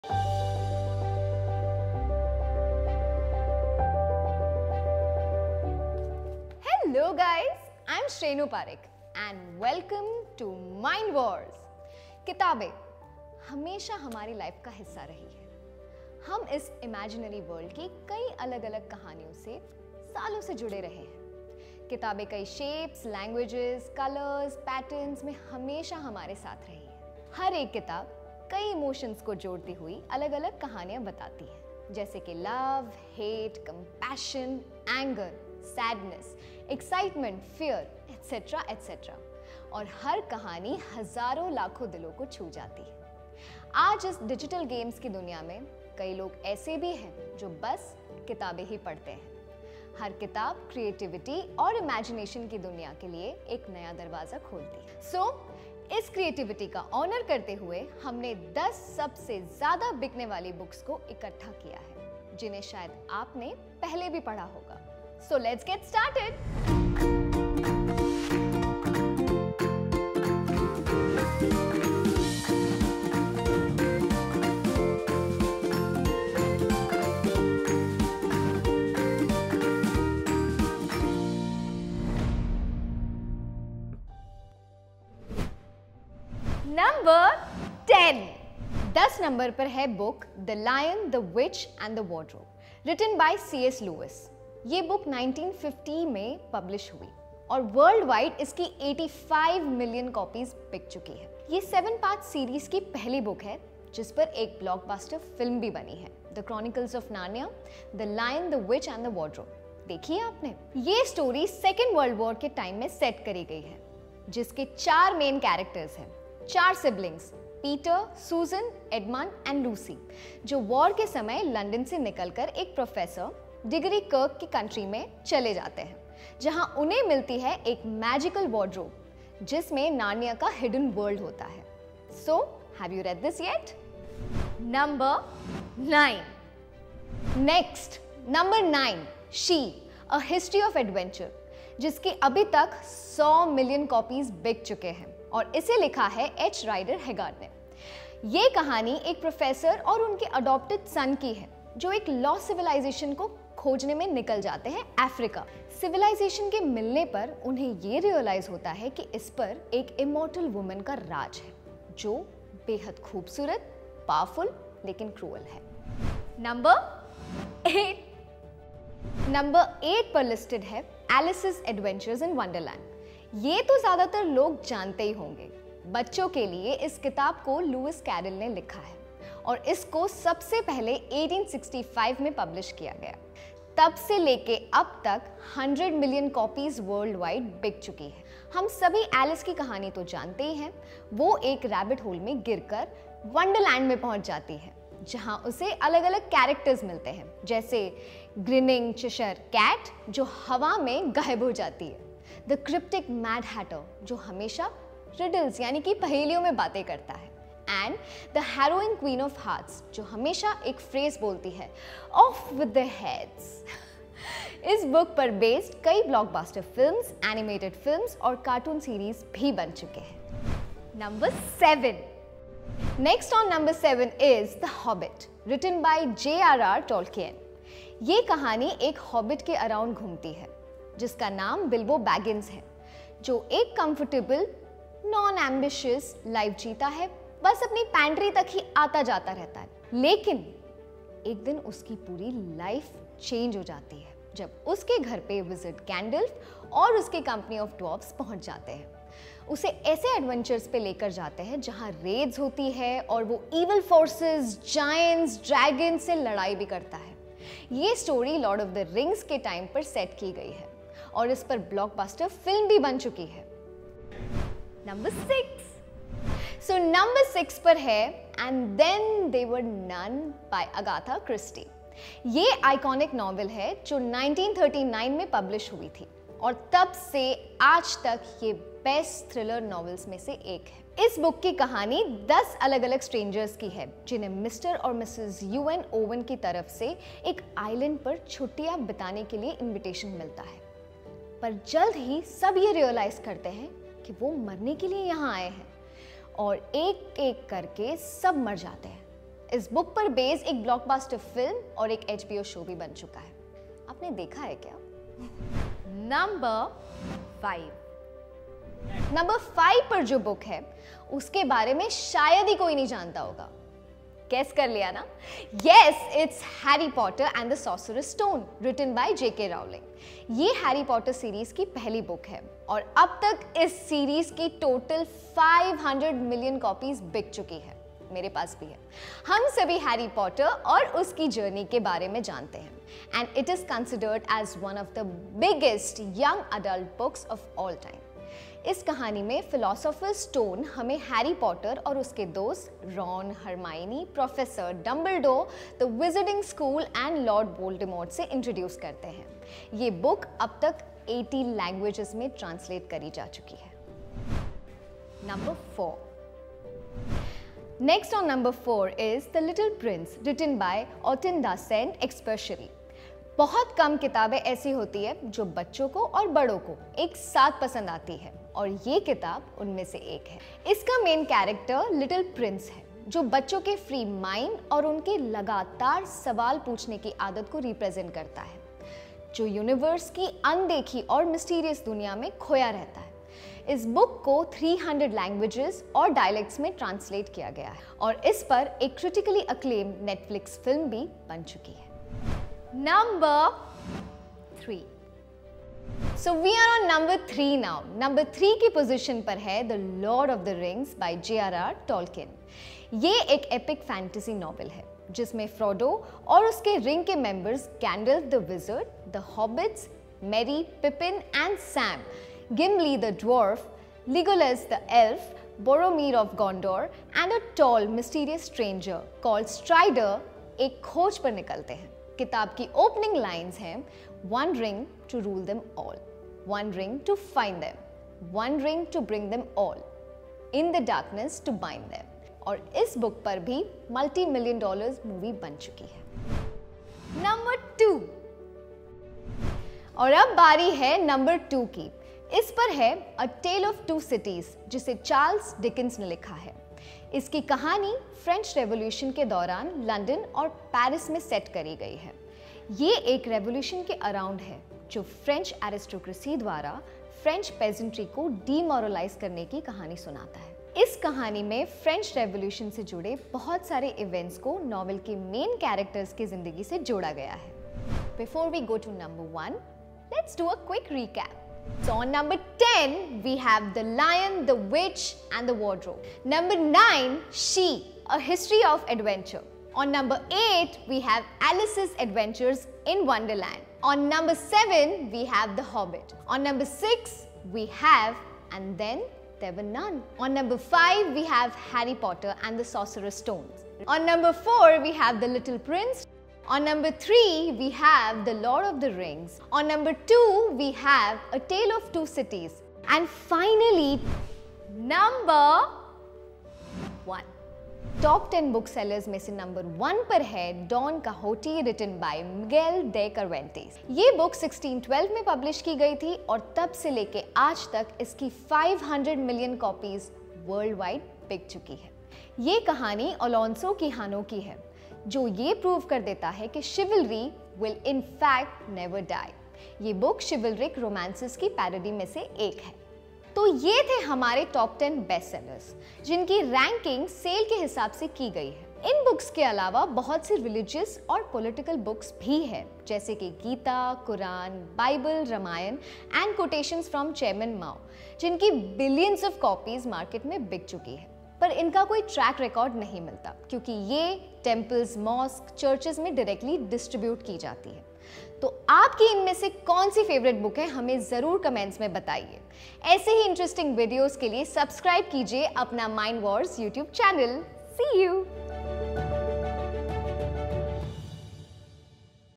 Top 10 World's Best Seller Books | Top 10 With Shrenu Parikh
Top 10 Best Seller Books
Books play a significant role in our lives. They are a blessing to mankind. They are packed with knowledge, insights into living a happy life, life lessons, love, fear, prayer, and helpful advice. Books have always been there, in different sizes, languages, colors, and patterns. Every book written since the beginning of time tells a different tale tangled in different emotions, such as love, hate, compassion, anger, sadness, excitement, and fear.
Different emotions, and different genres; each tale of fiction and nonfiction has touched millions of hearts. Even in today's era, when the majority is inclined to video games and online streaming, there are millions of people who still prefer to read. Every book is an escape from reality; every book is a new door to the world of creativity and imagination.
To honor this creativity and adventure, we have assembled the 10 best-selling books of all time, some of which you may have already read. So let's begin the journey.